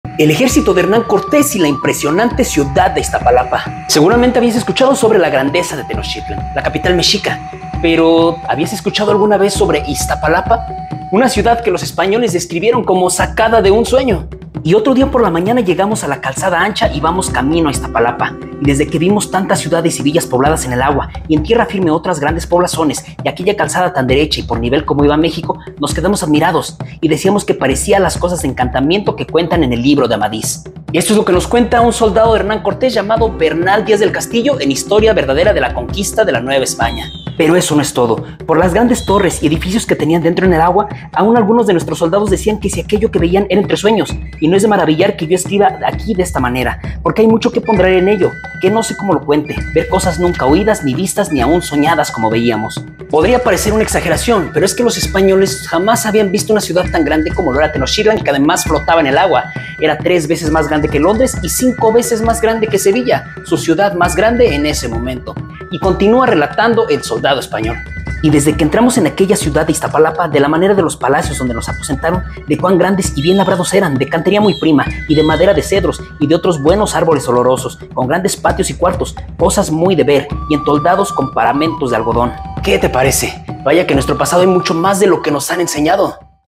El ejercito de Cortés y la impresionante ciudad de Iztapalapa
Tal vez has escuchado sobre Tenochtitlan pero ¿alguna vez escuchaste sobre Iztapalapa?